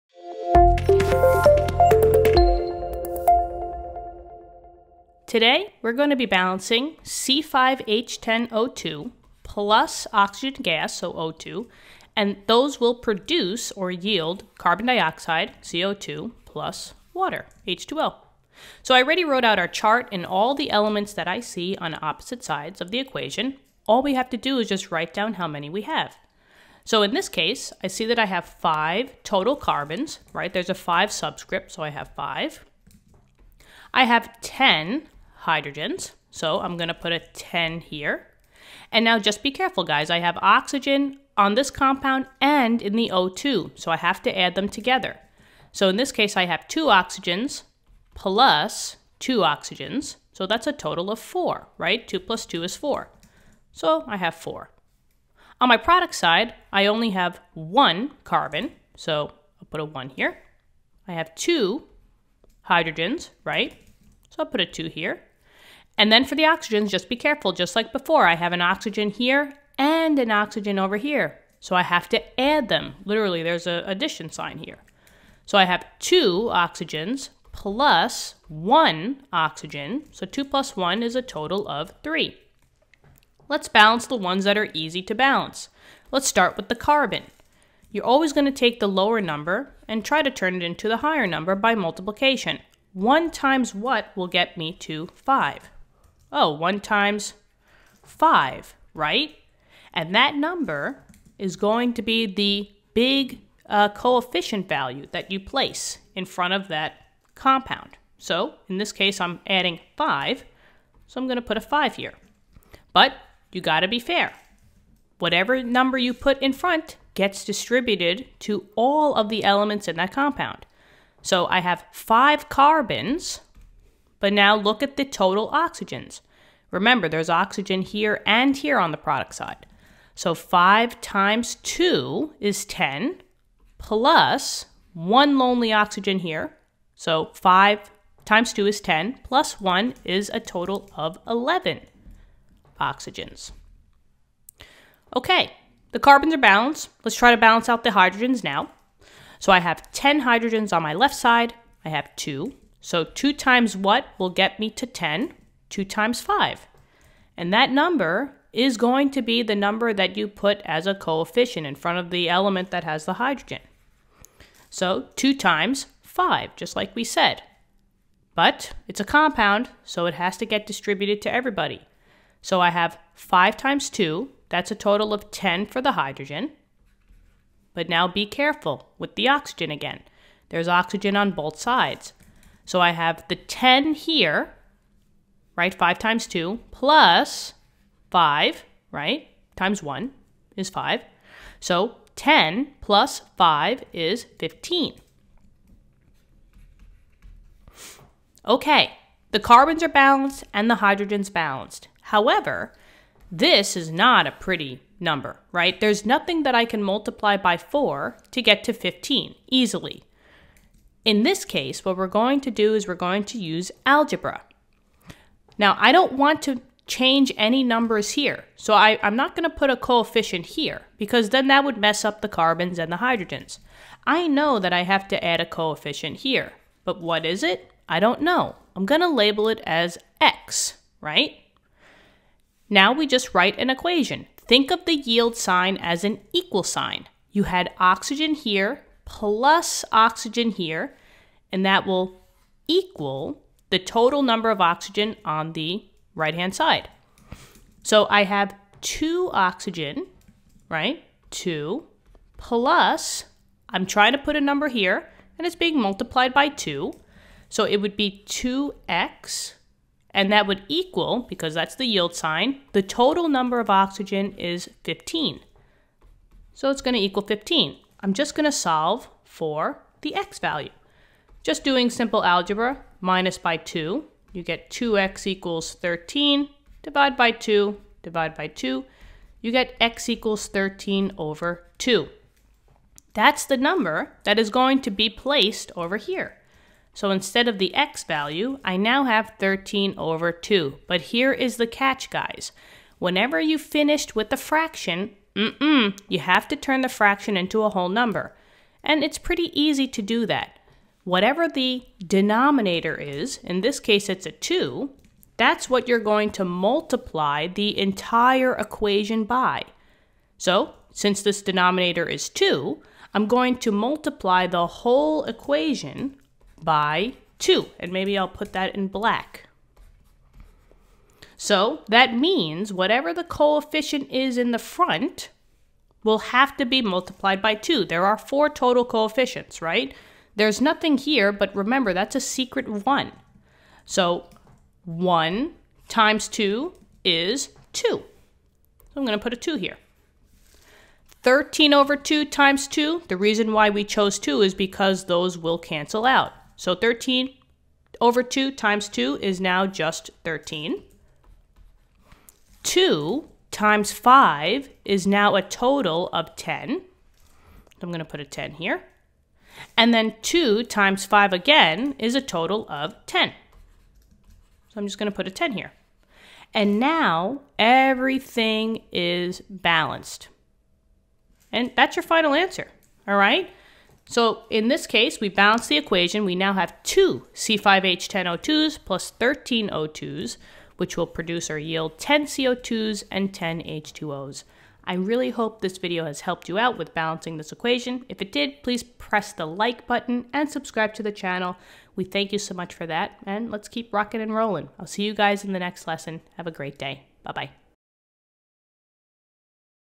How to balance C5H10O2(l) + O2(g) → CO2(g) + H2O(g)
Balance the following equation:
C5H10O2(l) + O2(g) → CO2(g) + H2O(g)